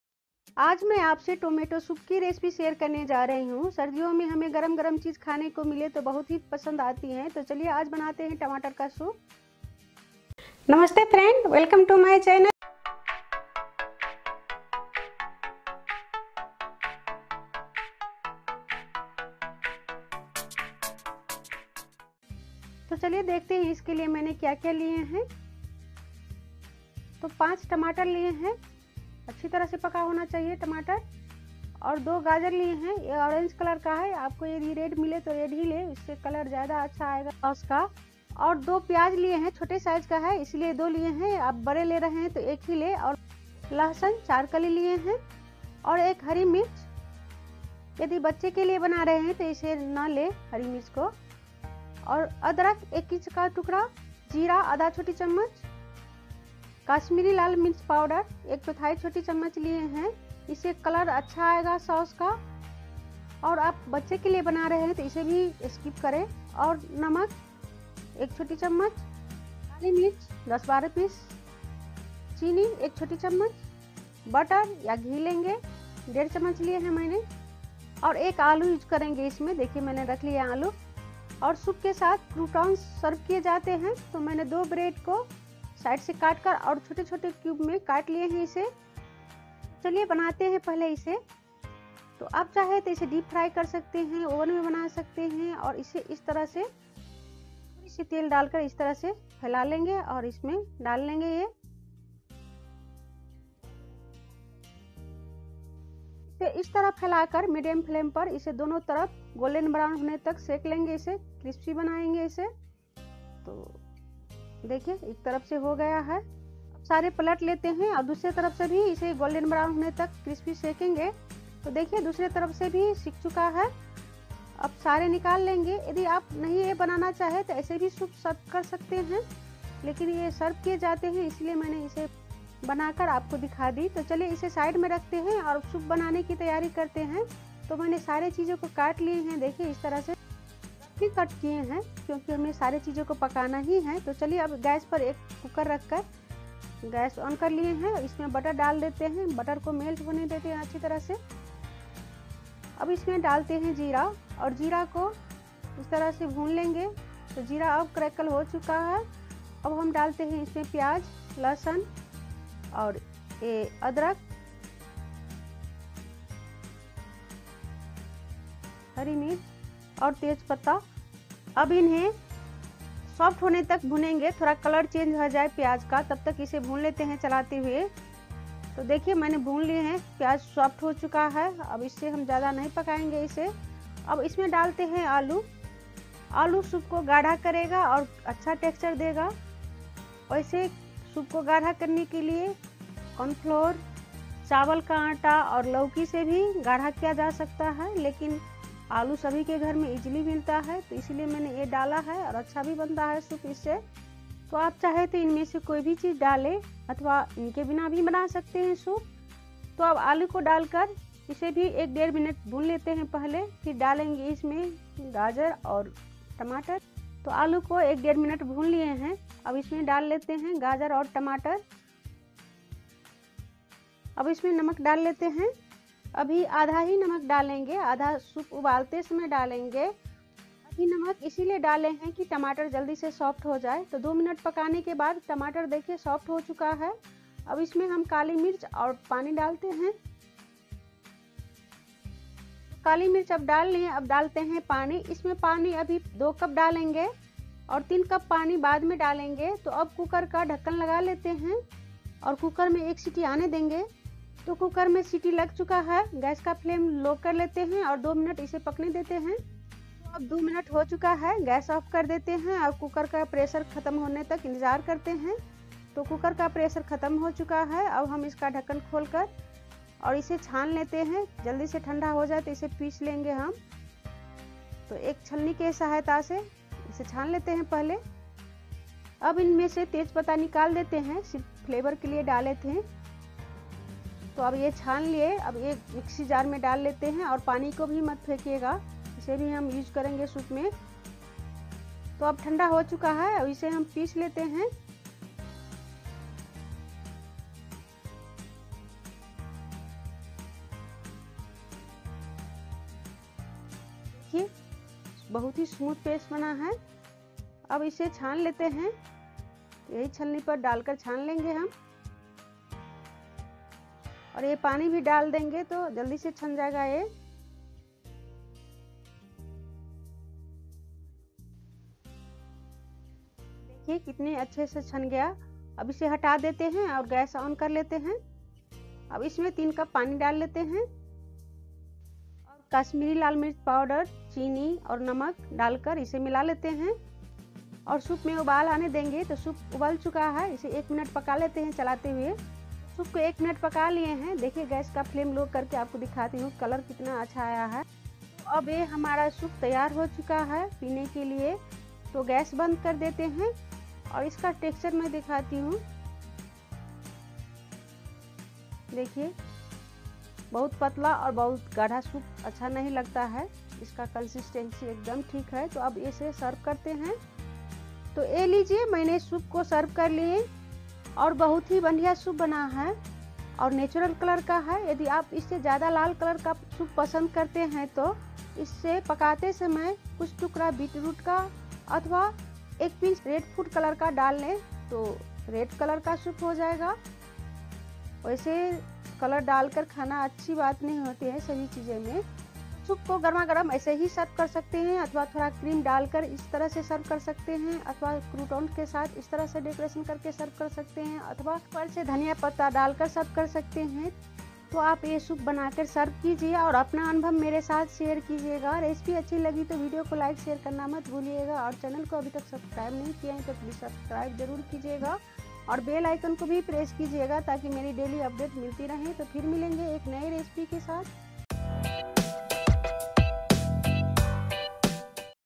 टमाटर सूप की विधि असली रेस्टोरेंट तरीका/ Perfect Creamy Tomato Soup And Croutons Recipe/ Soup Recipe
Ingredients:

Green chilli -1 pc
Potato – 1 pc
Black Pepper – 10-12 pcs
Kasamiri Red Chilli Powder – ¼ tsp
Salt – t tsp or as per taste

II LET FOOD BIND US ALL II